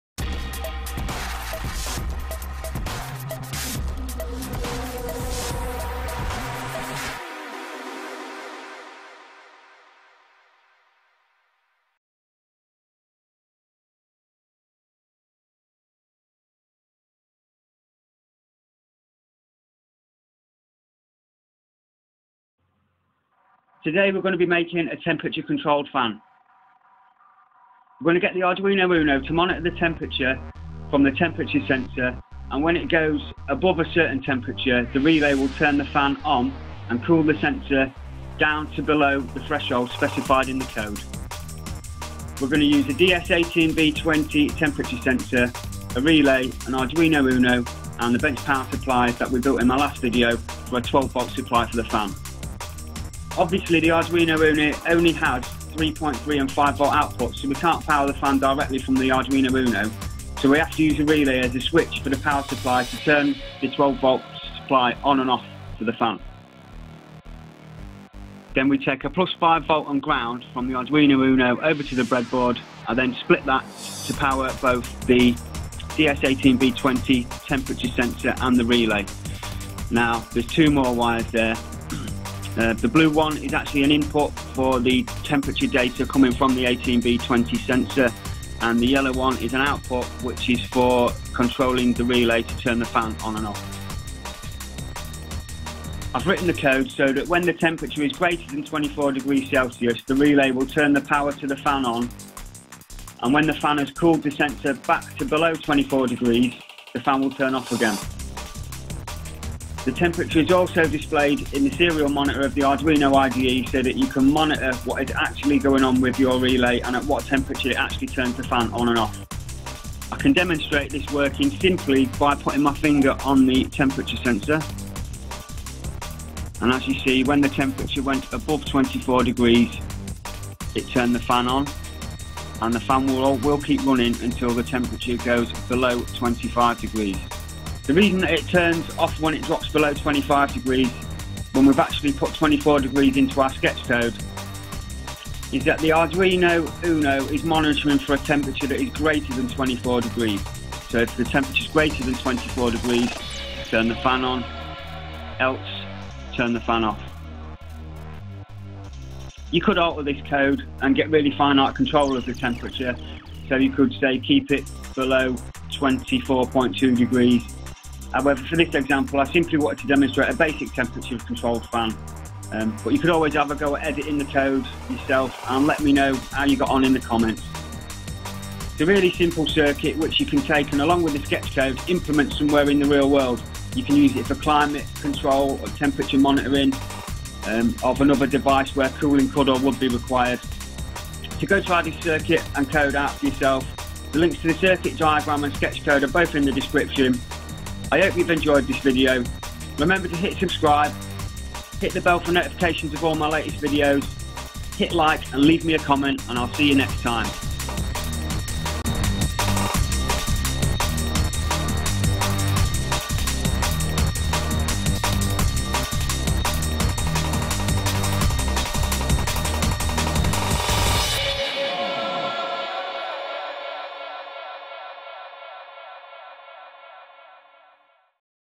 How To Make A Temperature Controlled Fan Using An Arduino Relay & Temp Sensor - ArDweNOme -
How To Make A Temperature Controlled Fan Using An Arduino UNO, A Relay & Temp Sensor - ArDweNOme -

This Video Shows You How To Make
A Fan Which Is Controlled Using A Temperature Sensor & Relay.

The Arduino will monitor the temperature of the sensor and when the temperature rises above a specified threshold it will activate the relay and turn the fan on to cool the sensor.
It's a very simple circuit and the code isn't too complicated either.

To Complete This Project You Will Need:

1 x Arduino Uno
1 x DS18B20 Temperature Sensor Module
1 x Relay Module (Capable Of Handling A Minimum Of 12 Volts!)
1 x 12 Volt DC Fan
1 x 12 Volt DC Power Supply
1 x Bread Board
6 x Jumper Wires